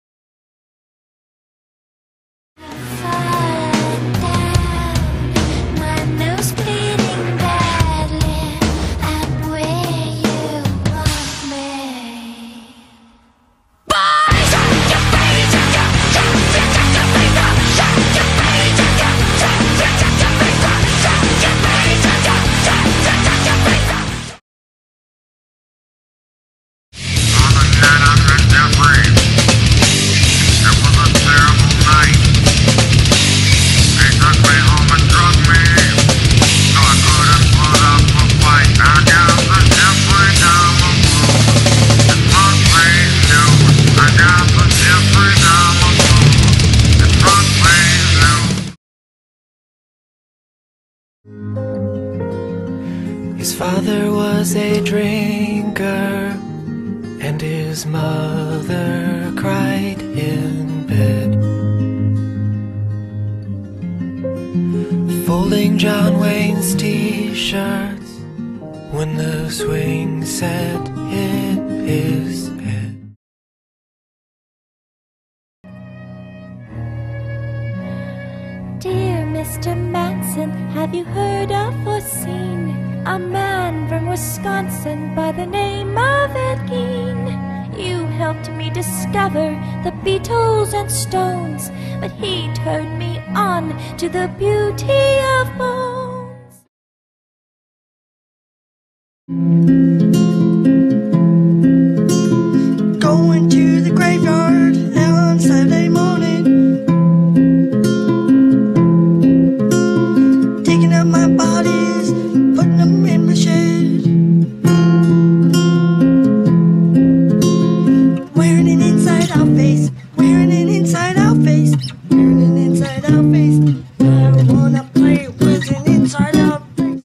The Killer-compilation
Something that I was originally gonna use for Digital Arts-class when we were working with flash. The assignment was to make a flash-navigation, and I thought I'd make something about serial killers. Therefore, I edited together these music-videos (or whatever you wanna call them) to add on every page in my navigation about respective killer.
Unfortunately, I had too much confidence in my own ability with Flash, so everything screwed up in the end. Since I didn't wanna let these little babies go to waste, I figured I'd just put them all into one video and upload them here!
So, there you go! Enjoy!